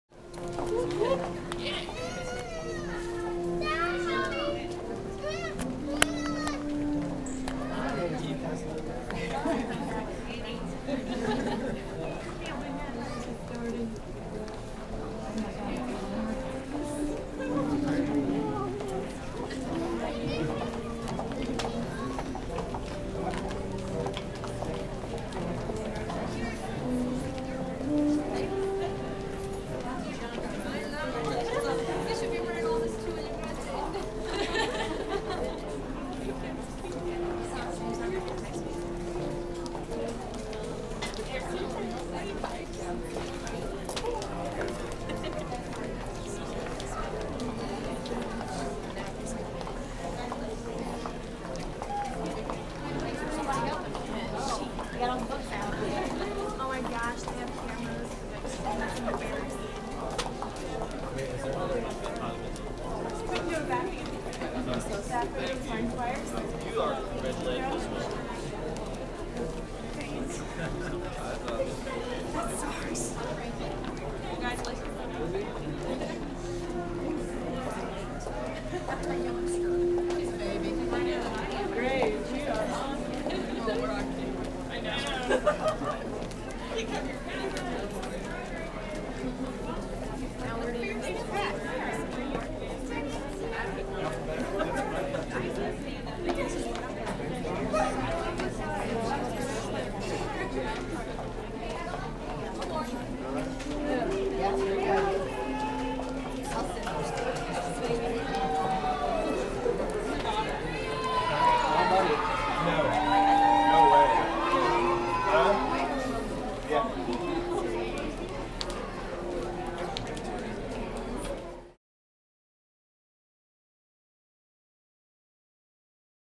East Central College 2010 Graduation
This gallery contains pictures from East Central College graduation ceremony Thursday, May 21, at the campus in Union. Missouri Gov. Jay Nixon addressed the graduates. More than 430 students were candidates for graduation at ECC.